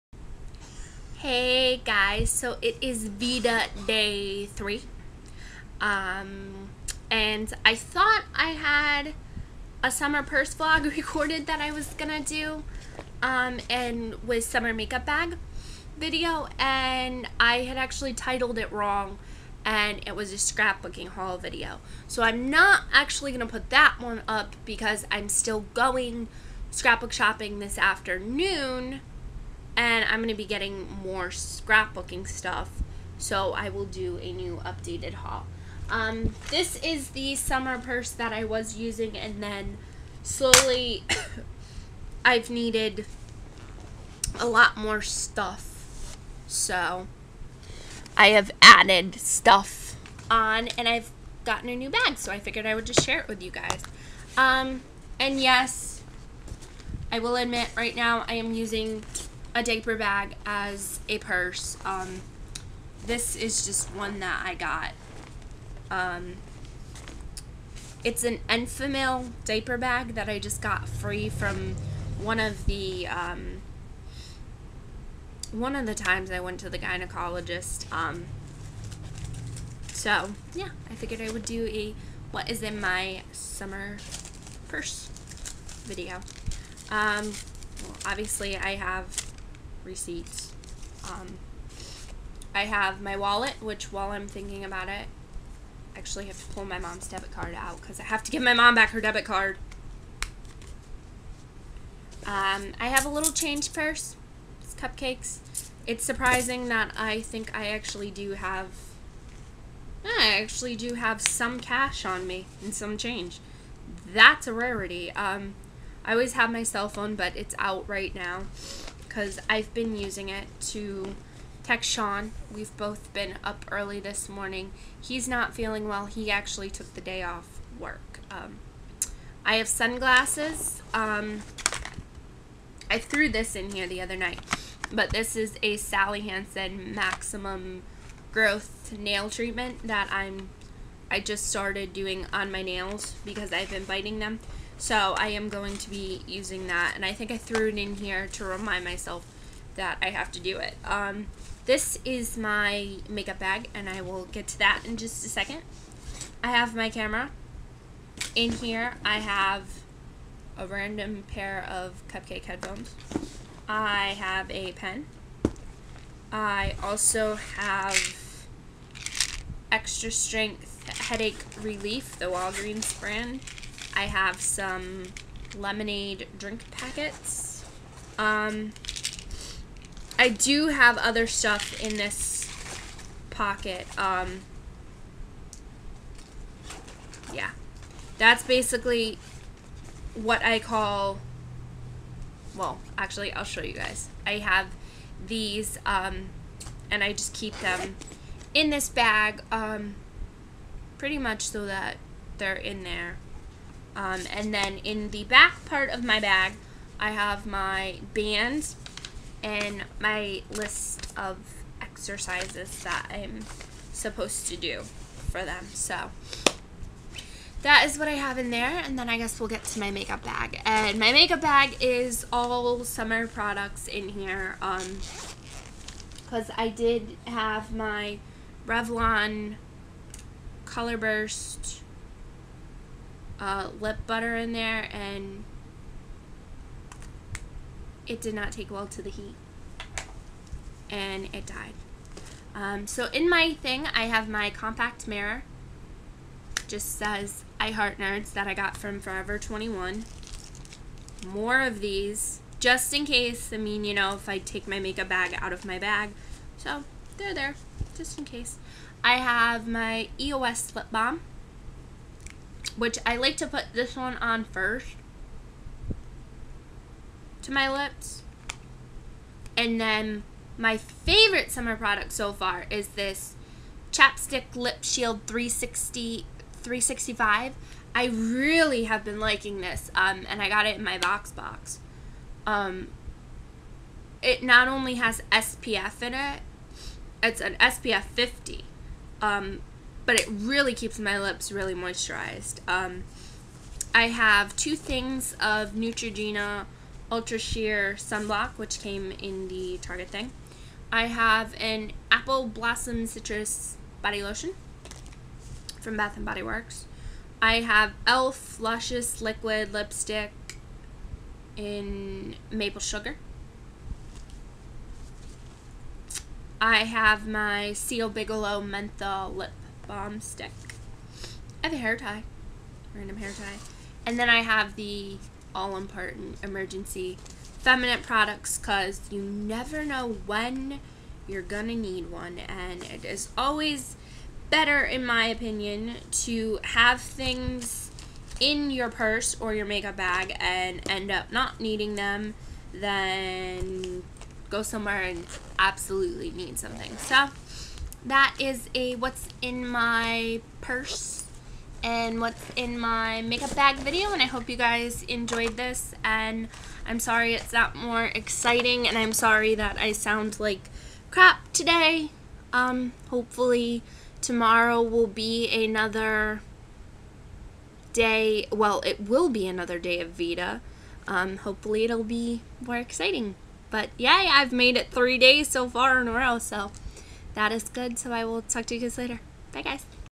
Whats in my purse and makeup bag (VEDA#3)
Showing all the stuff in my purse and makeup bag